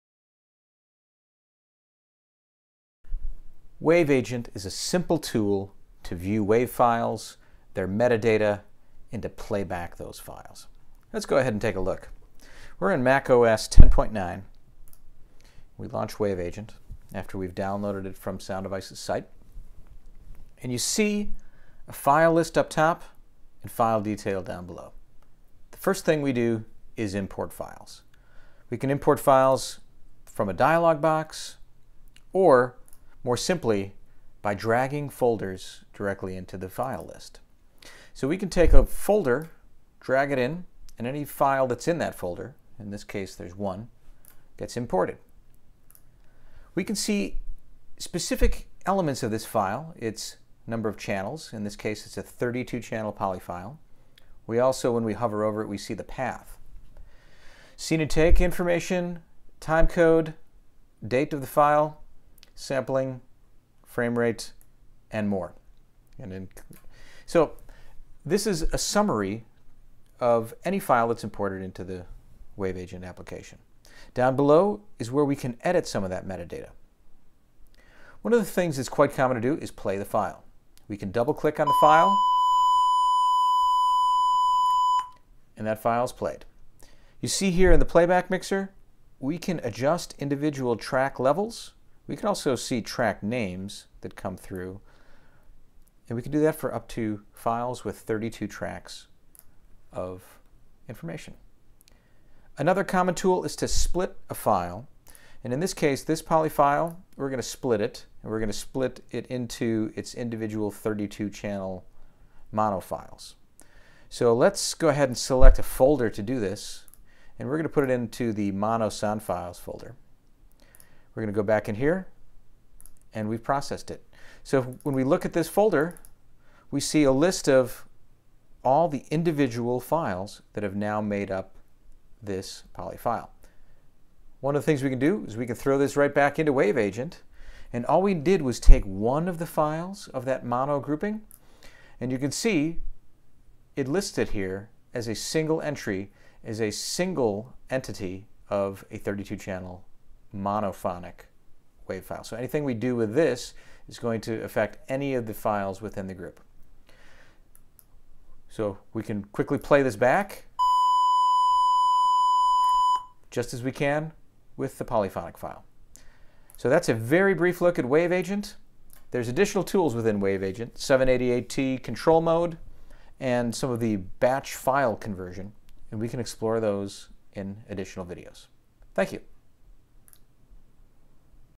Sound Devices Wave Agent 1.20 Overview
Wave Agent is Sound Devices Mac OS and Windows WAV file utility. It offers multi-track playback (up to 32-tracks), metadata editing, and poly/mono split/combine utilities.